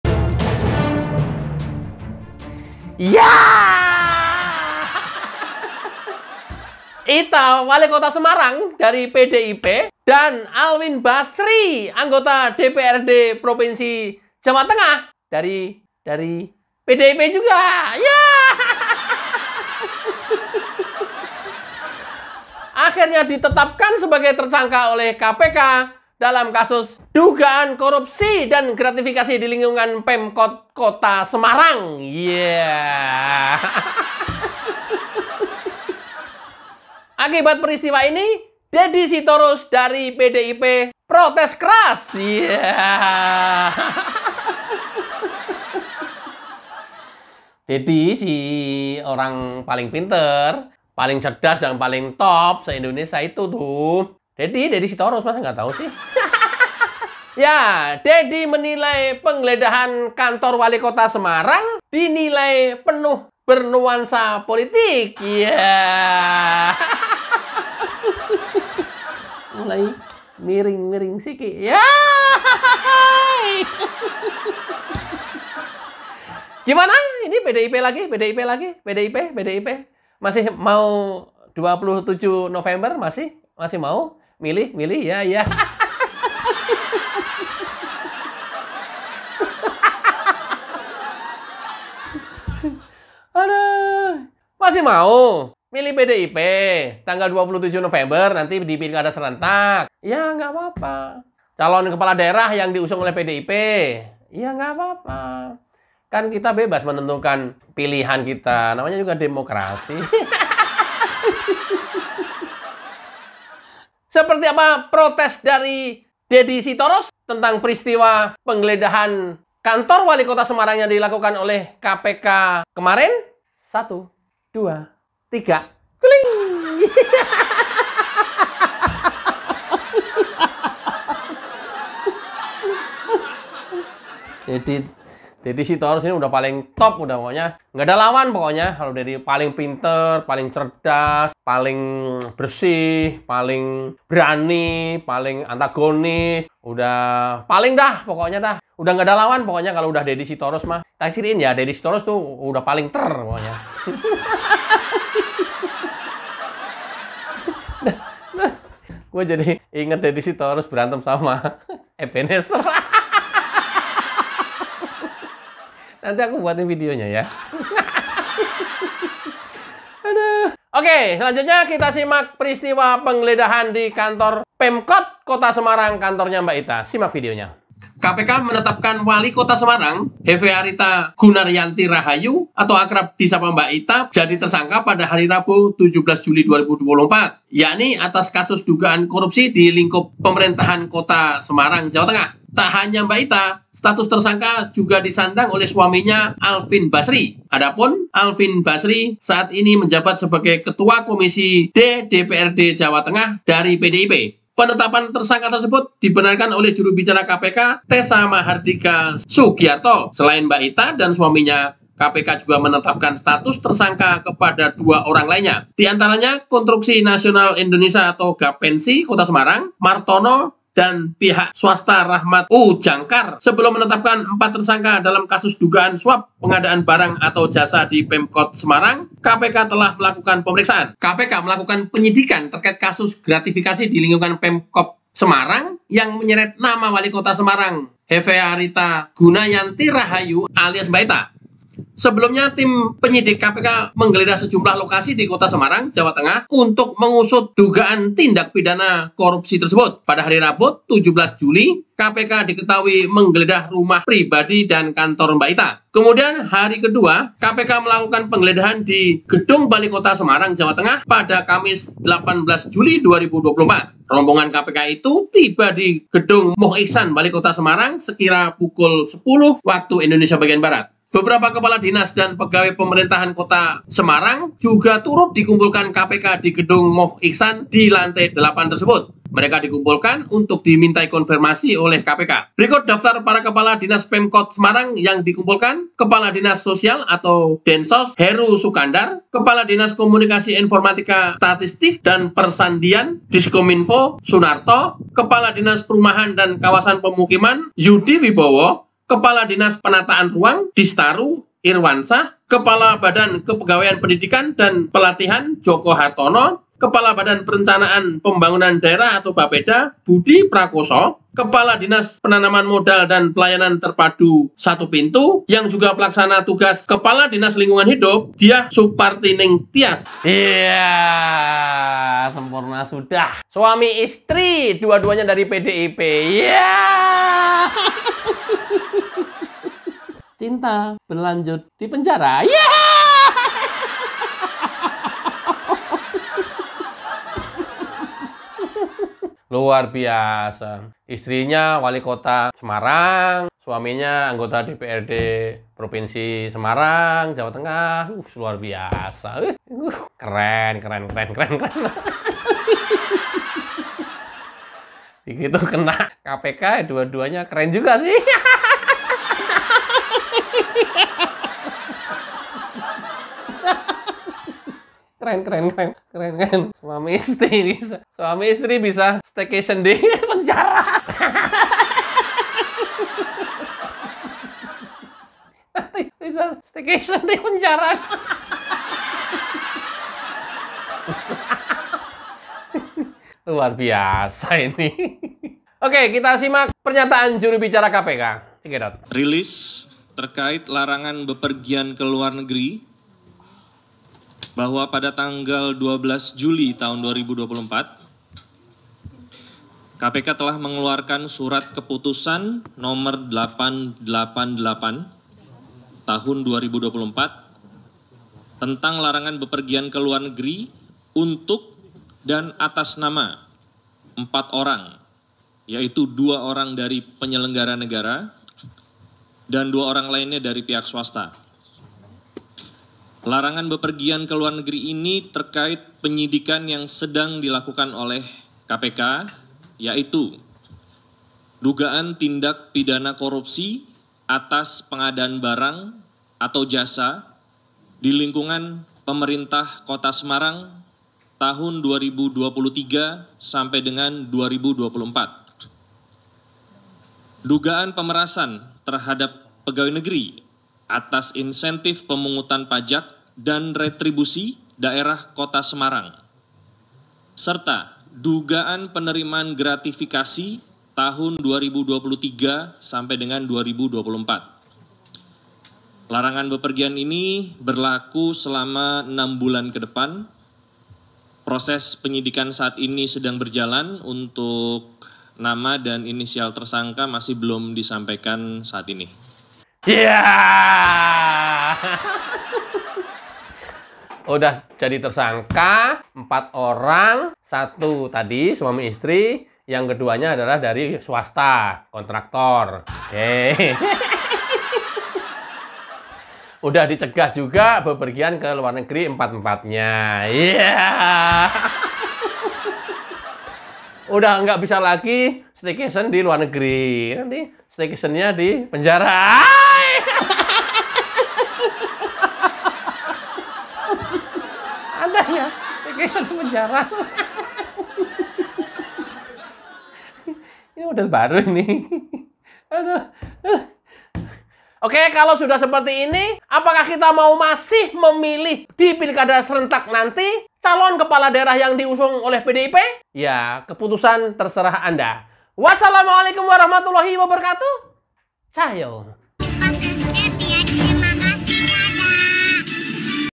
Wali Kota Semarang Mbak Ita & Suaminya Ditetapkan Tersangka oleh KPK, Begini Respons Keras PDIP
Komisi Pemberantasan Korupsi (KPK) menetapkan Wali Kota Semarang, Hevearita Gunaryanti Rahayu atau akrab disapa Mba Ita jadi tersangka pada Rabu (17/7/2024). Yakni atas kasus dugaan korupsi di lingkup Pemerintah Kota (Pemkot) Semarang, Jawa Tengah.
Tak hanya Mba Ita, status tersangka itu juga disandang oleh suaminya, Alwin Basri. Adapun Alwin Basri saat ini menjabat sebagai Ketua Komisi D DPRD Jawa Tengah (Jateng). Penetapan tersangka tersebut dibenarkan oleh Juru Bicara KPK, Tessa Mahardika Sugiarto.
Selain Mba Ita dan suaminya, KPK juga menetapkan status tersangka kepada dua orang lainnya. Di antaranya Konstruksi Nasional Indonesia (Gapensi) Kota Semarang, Martono dan pihak swasta Rahmat U Djangkar.
Sebelum menetapkan empat tersangka dalam kasus dugaan suap pengadaan barang atau jasa di Pemkot Semarang, KPK telah melakukan pemeriksaan.